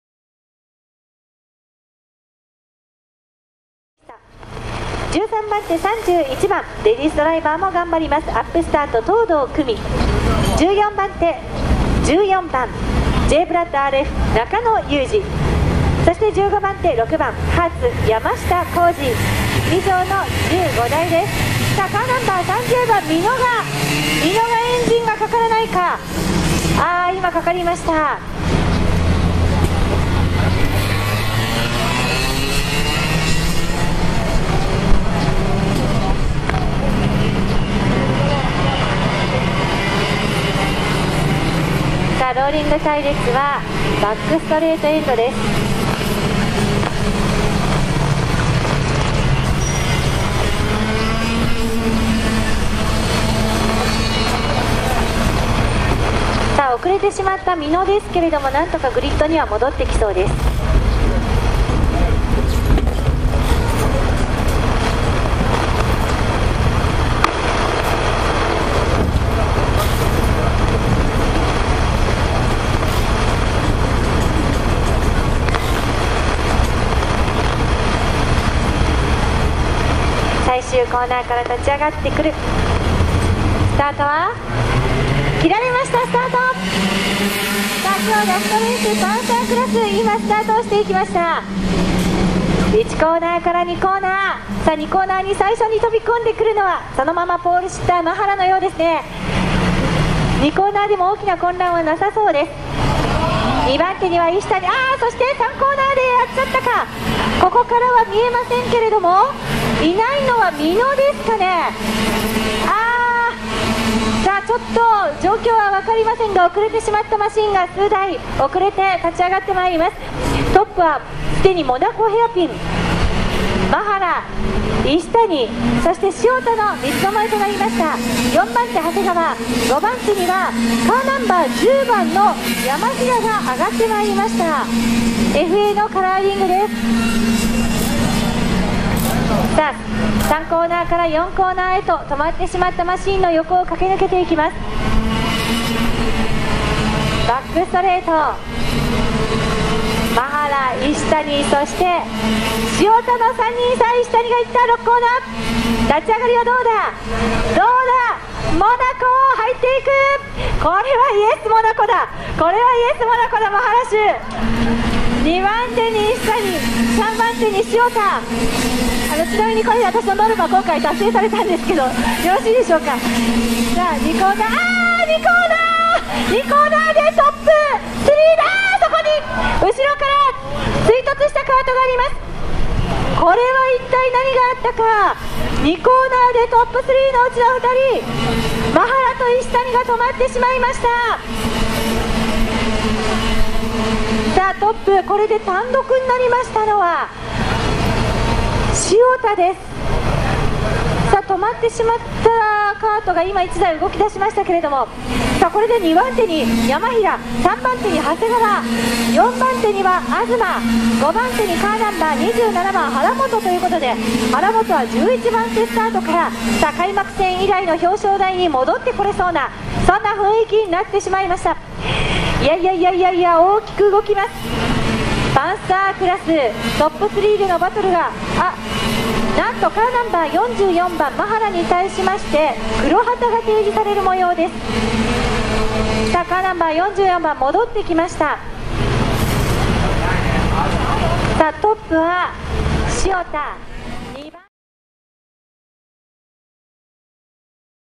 2015.05.10 NTC CUP Rd.3 パンサークラス 決勝ヒート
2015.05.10、新東京サーキットで行われたNTC CUP 第3戦 パンサー 決勝ヒートの模様です。
MPRドライバーは真原愁。

この動画は、レーシングドライバー光貞秀俊率いるレーシングカートチーム「MITSU­SADA PWG RACING（MPR）」の公式レース動画です。